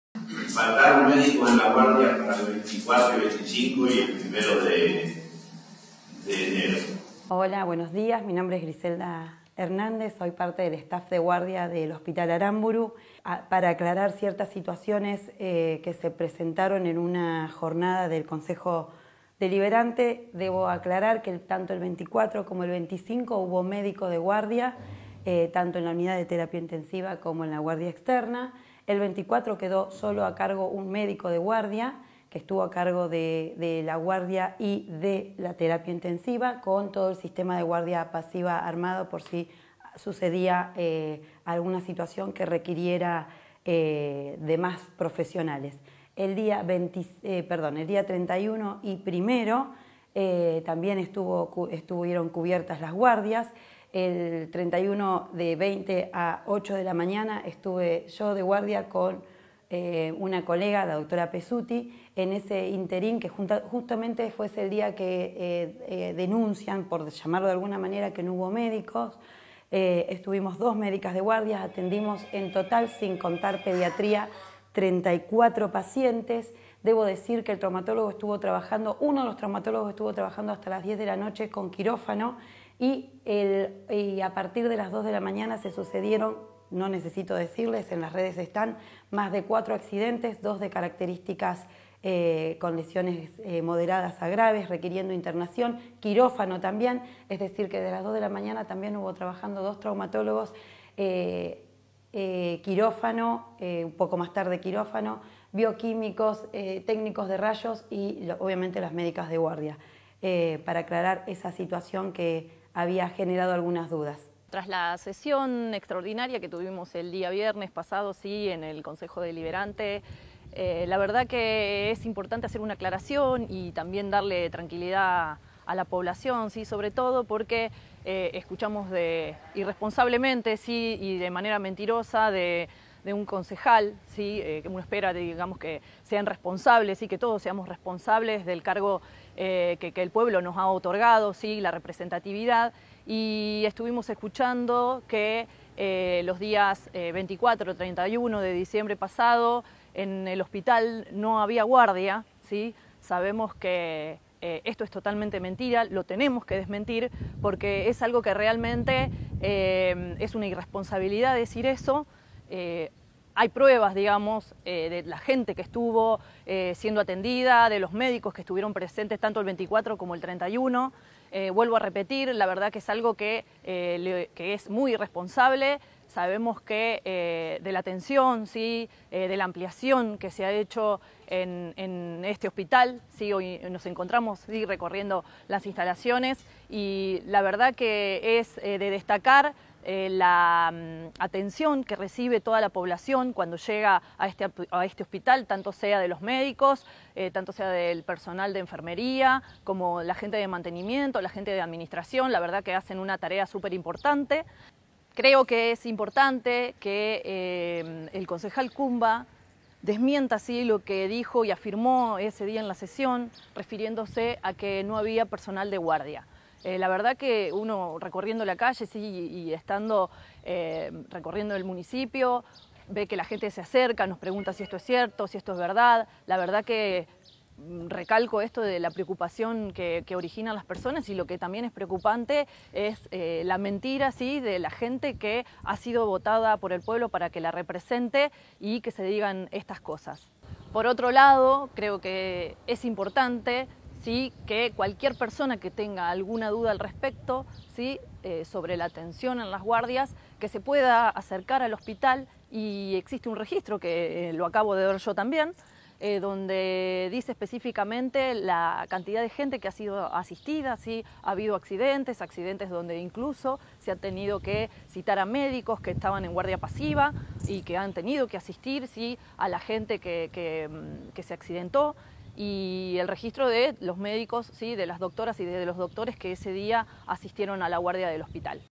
“Es importante que el Concejal Cumba desmienta lo que dijo" (Julieta Martín)
La Dra. Griselda Hernández y la Concejal Julieta Martín respondieron a los dichos del Concejal Juan Miguel Cumba durante la Sesión Extraordinaria del HCD.
El Concejal, Juan Miguel Cumba, en una parte de su alocución, expresó que durante los días 24 y 25 de diciembre y el 1 de enero faltaron médicos en la guardia del Hospital Pehuajó, lo que motivó que la Dra. Griselda Hernández y la Concejal Julieta Martín salieran a desmentir tal aseveración.
La Dra. Hernández aclaró que todos los días mencionados hubo médicos de guardias tanto en la Unidad de Terapia Intensiva como en la guardia externa, con todo el sistema de guardia pasiva armada por si sucedía alguna situación que requiriera de mas profesionales.
Además aclaró que hubo traumatólogos trabajando, que hubo cuatro accidentes que requirieron internación y quirófano, bioquímicos y técnicos de rayos.
La Concejal, Julieta Martín, expresó que “Escuchamos irresponsablemente y de manera mentirosa de parte de un Concejal, que hubo días que en el Hospital no hubo guardia. Esto es mentira y lo tenemos que aclarar”.

Más adelante dijo “Es importante que el Concejal Cumba desmienta lo que dijo y afirmó, refiriendo que no había personal de guardia en el Hospital, es preocupante la mentira de la gente que ha sido votada para que lo representen”.

Sobre el final, Julieta Martín, le pidió a los vecinos que tengan alguna duda sobre la atención en la guardia, que se acerque al Hospital donde existe un registro donde dice específicamente la cantidad de gente que se atiende. los médicos que están de guardia y el registro de los médicos que están en guardia pasiva y son llamados por urgencias.